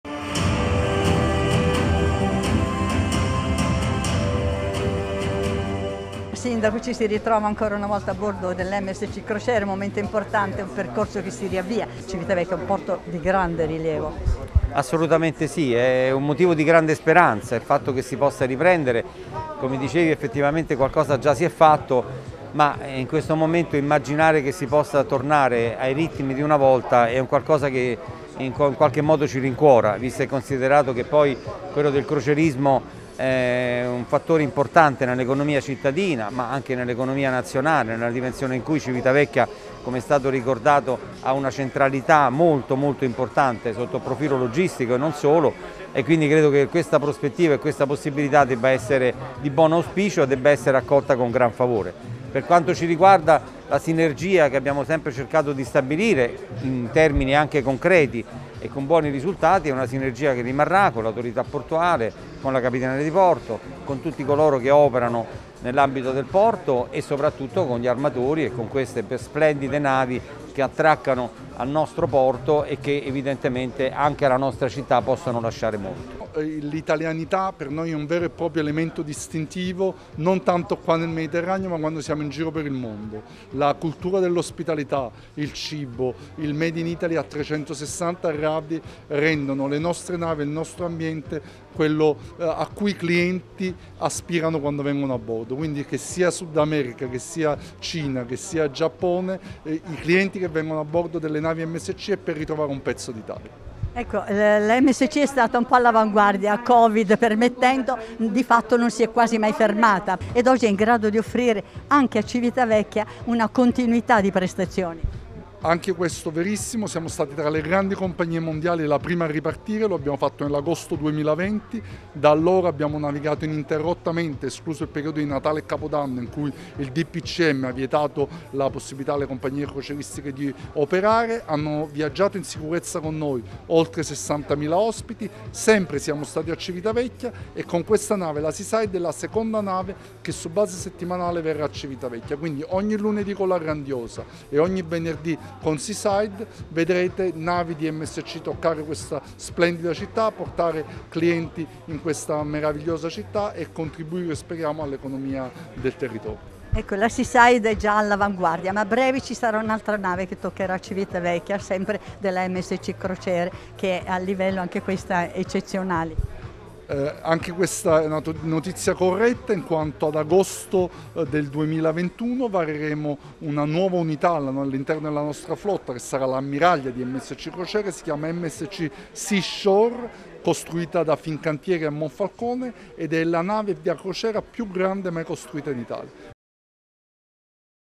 Il Porto dà il benvenuto a Msc Seaside
Il porto ha dato il benvenuto a MSC Seaside, nave tra le più avanzate al mondo dal punto di vista tecnologico, che farà scalo ogni venerdì per l’intera stagione estiva.